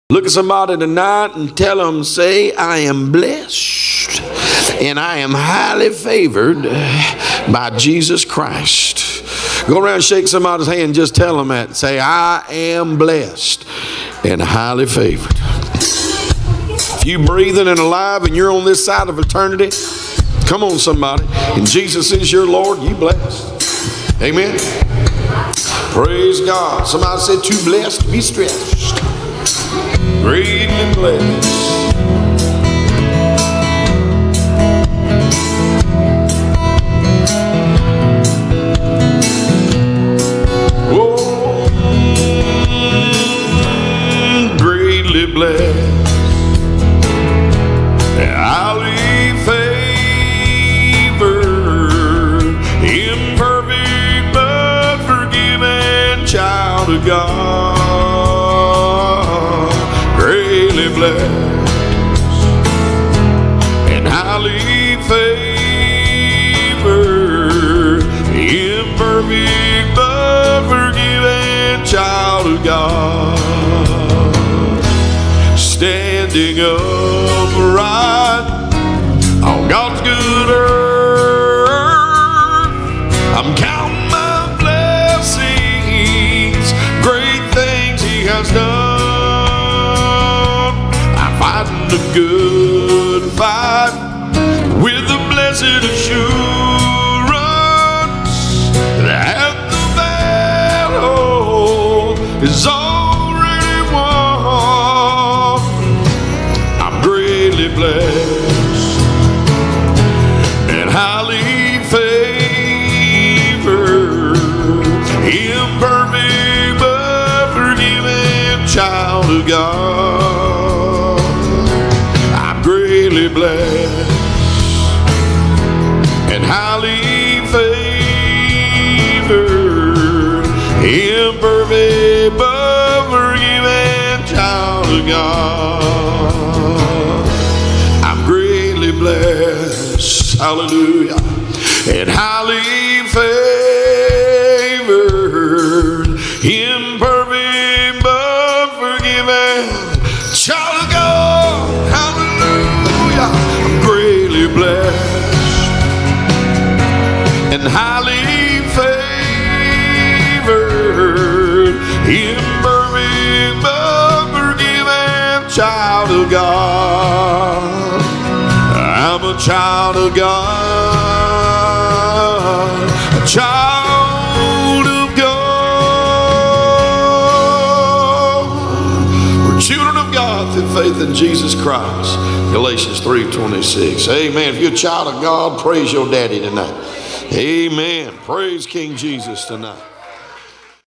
"Greatly Blessed and Highly Favored" *live*
Pastor Marvin Booth sharing in praise to God how the believer is greatly blessed and highly favored. Recorded live at "Acts 29 Church unto God", in Waycross Ga.